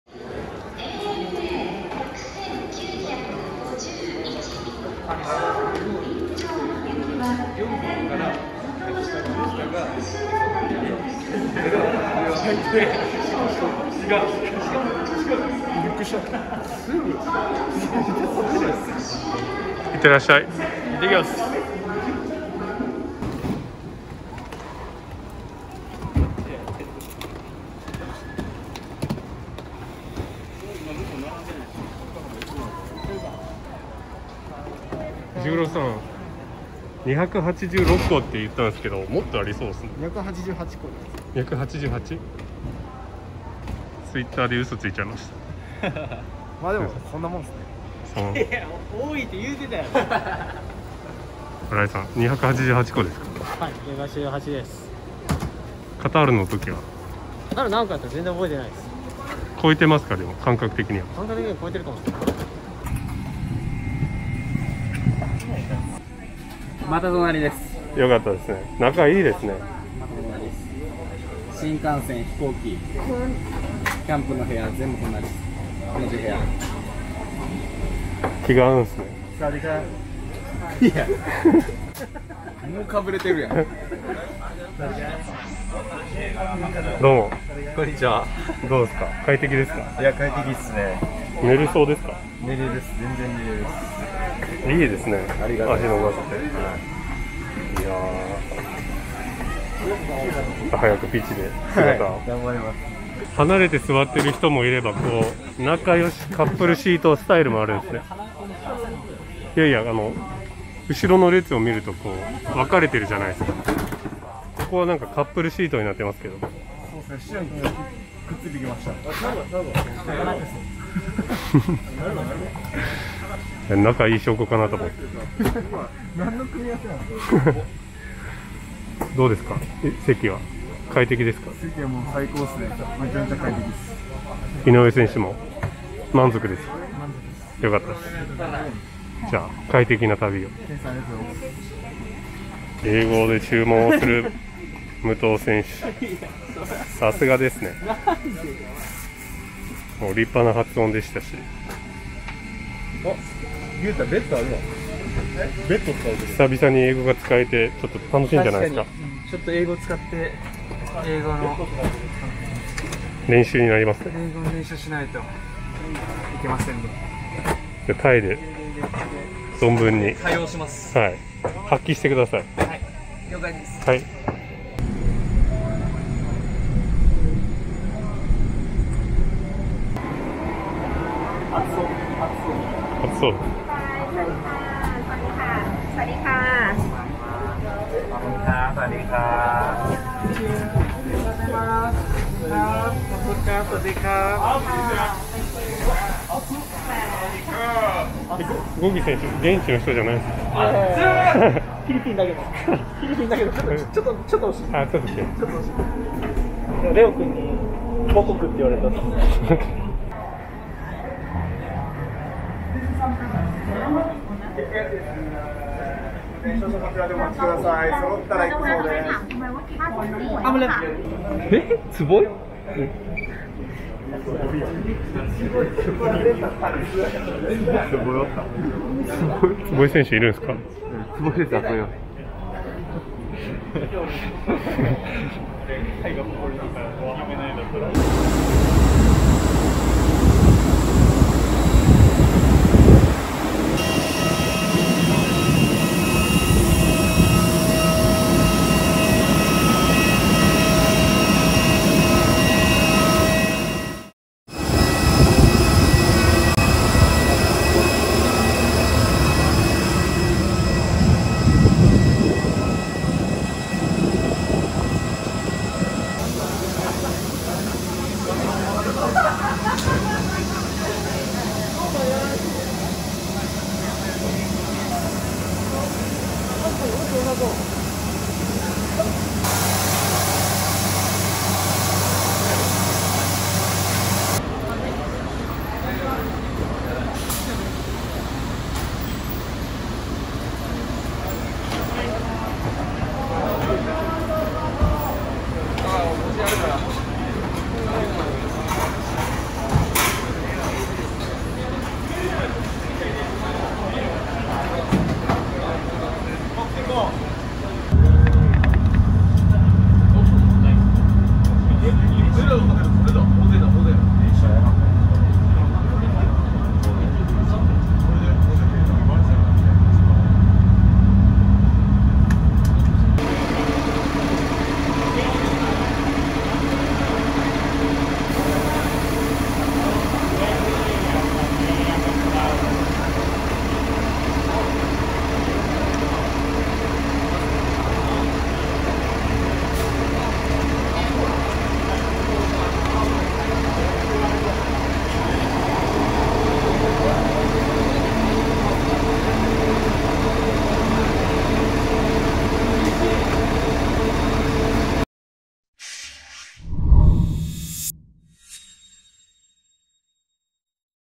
［神戸からタイへ移動！］ACLグループステージ DAY1【VISSEL KOBE】
タイ・ブリーラムで行われるACLグループステージに向けて、日本を出発！
初日の様子をお届けします🎥